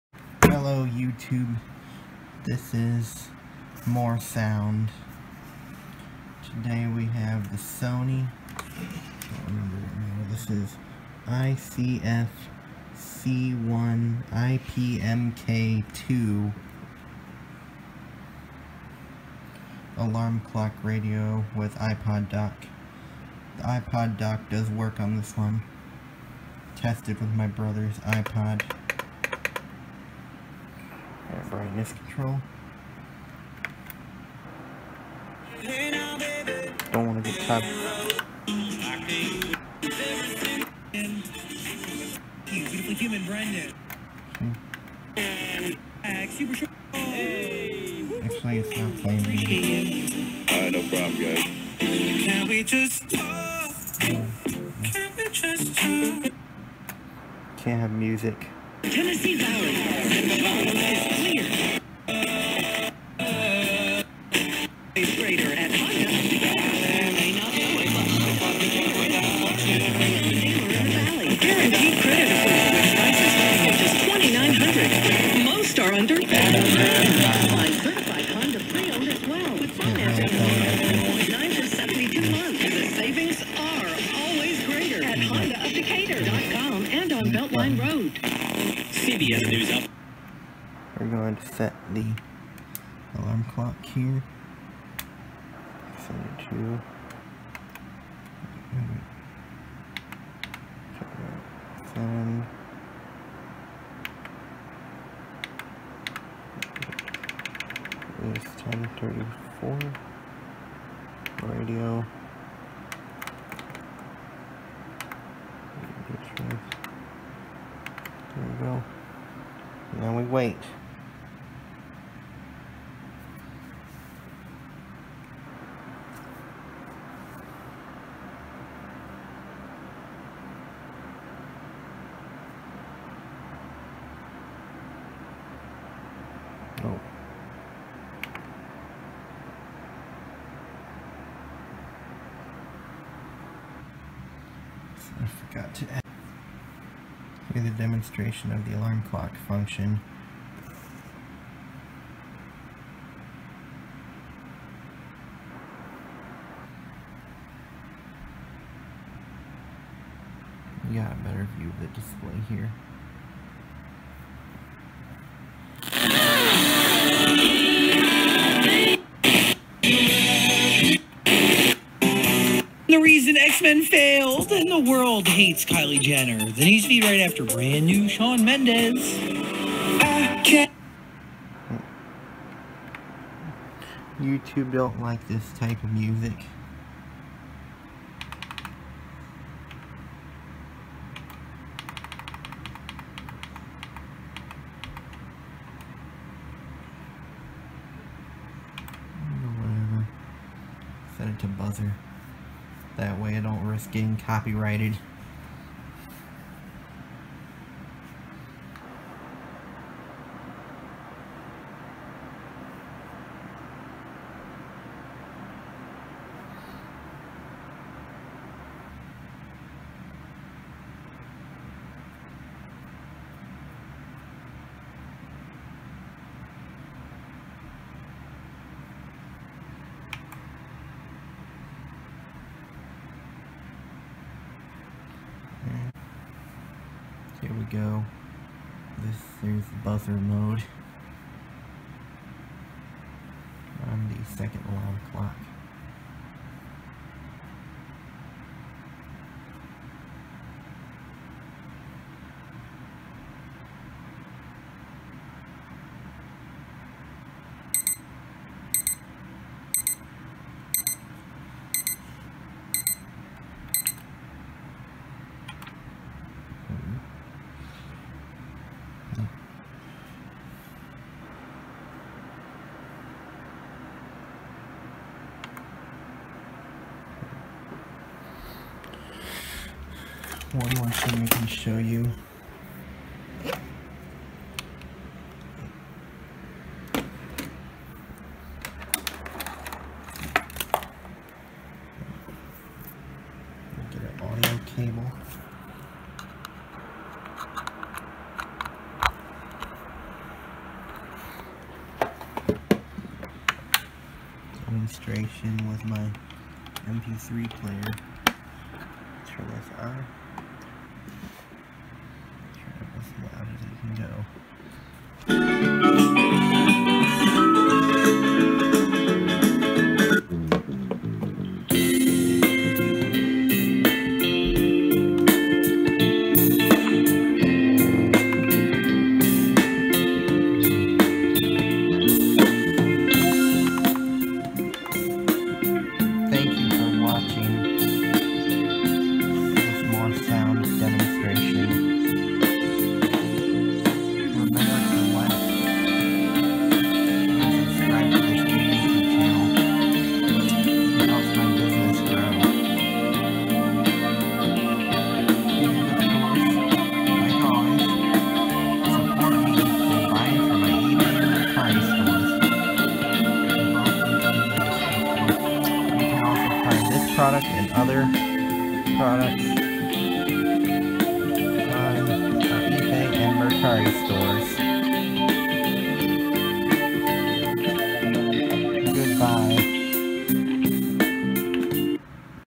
(Sold out) Sony Dream Machine ICF-C1iPMK2 FM/AM Alarm Clock Radio iPhone iPod Dock w/Remote
Sorry, this product is no longer available.

Sony Dream Machine ICF-C1iPMK2 FM/AM Alarm Clock Radio iPhone iPod Dock w/Remote for sale.

This product is missing the remote control & AM antenna, but the device is in good condition.

Features/Specifications:

-Automatic Daylight Savings Time adjustment

-Dock for iPod 4 or earlier

-Dual alarm clock

-Aux input for audio devices

-AM/FM radio with 5 programmable memory buttons

-2-Way stereo speakers with Mega Xpand

Thank you for shopping at Moore Sound.